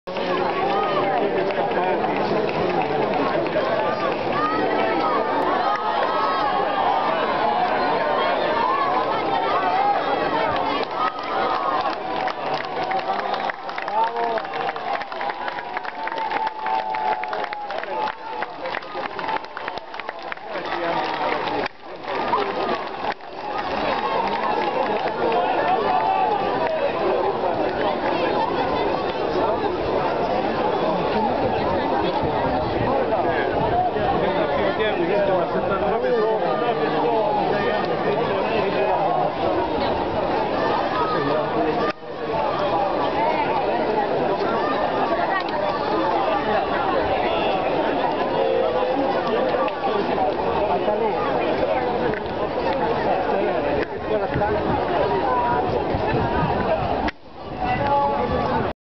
Roma 2010 - Paolo Lorenzi.avi
L'uscita dal campo di Paolo Lorenzi dopo aver battuto al primo turno lo spagnolo Albert Montanes sui campi del Foro Italico agli Internazionali d'Italia